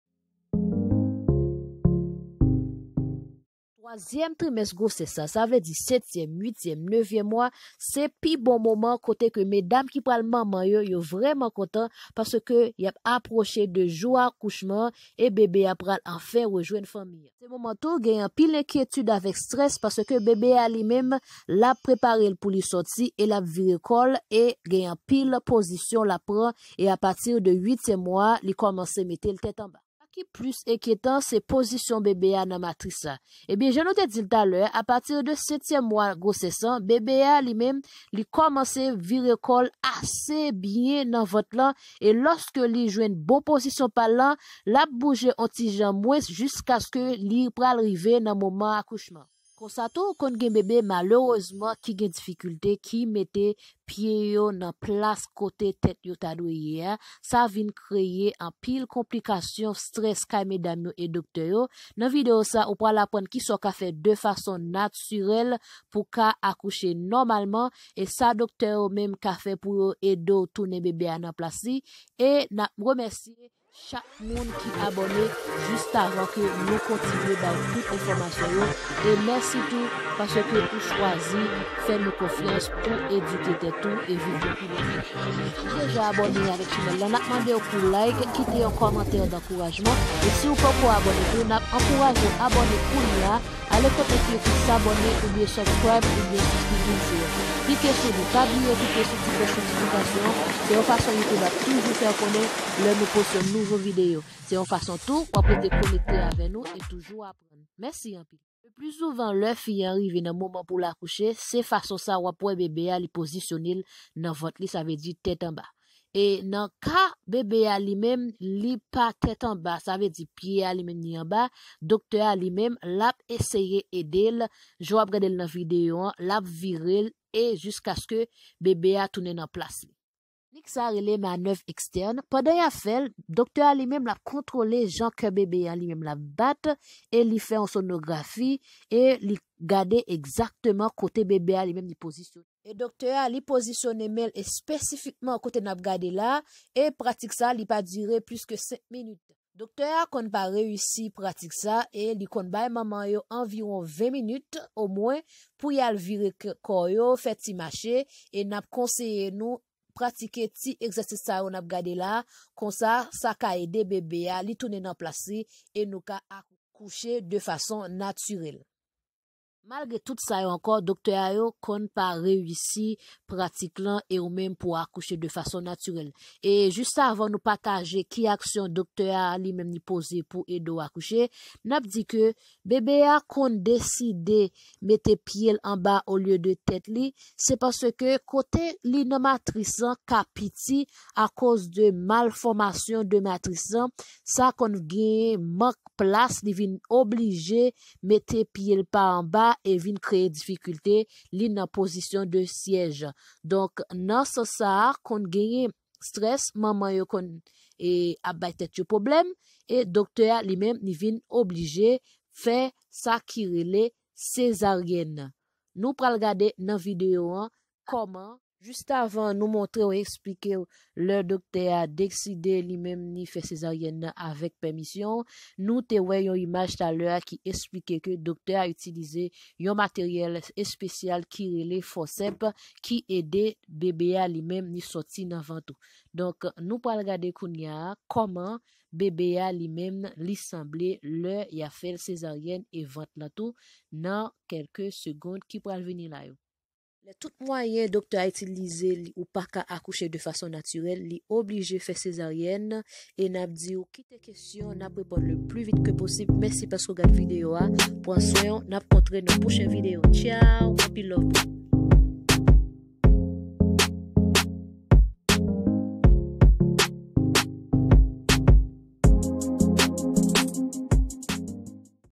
Sak fè fanm ansent fè sezaryèn? bebe chita Nan matris/ vire bebe tèt anba / bebe ki chita nan matris
Pa neglije kite kesyon e nap reponn ou pi vit posib/like e komante
Anpil lov!